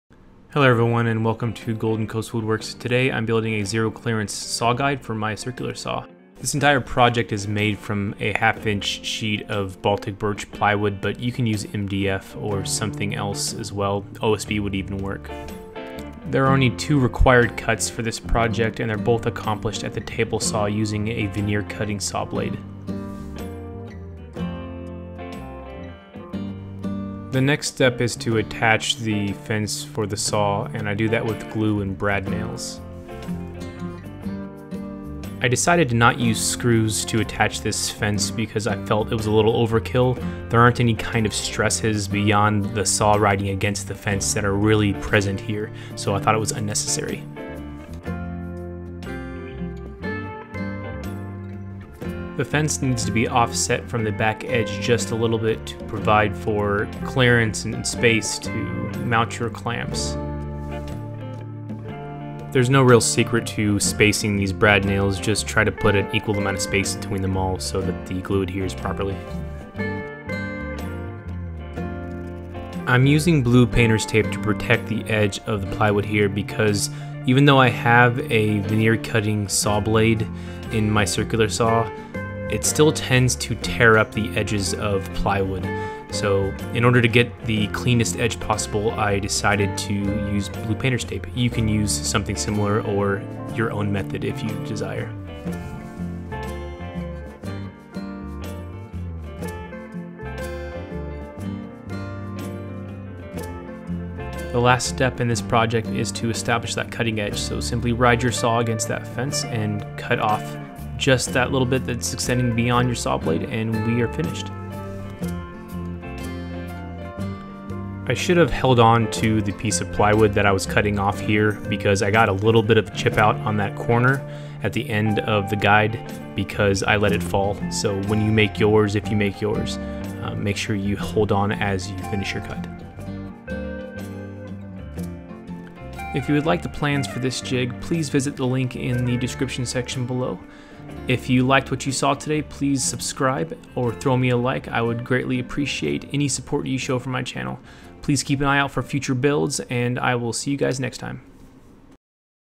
DIY Circular Saw Guide | Build One in a Few Simple Steps
Don't want to pay track saw prices? Having issues making straight cuts with your circular saw? This guide will give you the convenience that a track saw provides at a fraction of the price.